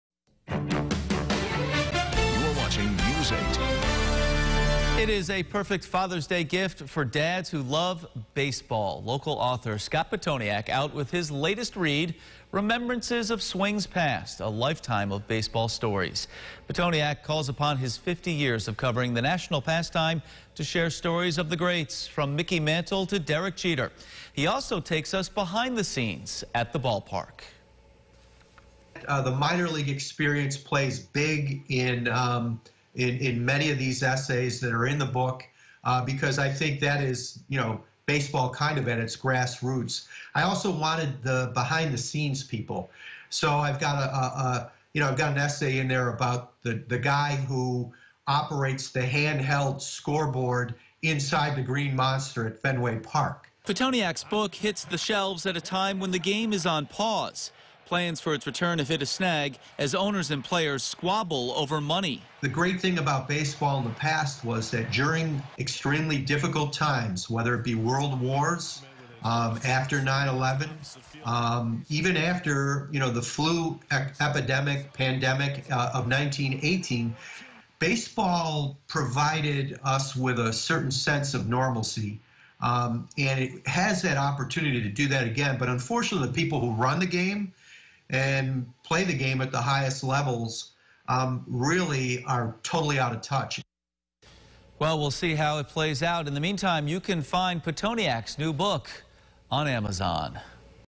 A summer read for baseball lovers
Local author Scott Pitoniak has published a new for baseball fans entitled "Remembrances of Swings Past: A Lifetime of Baseball Stories."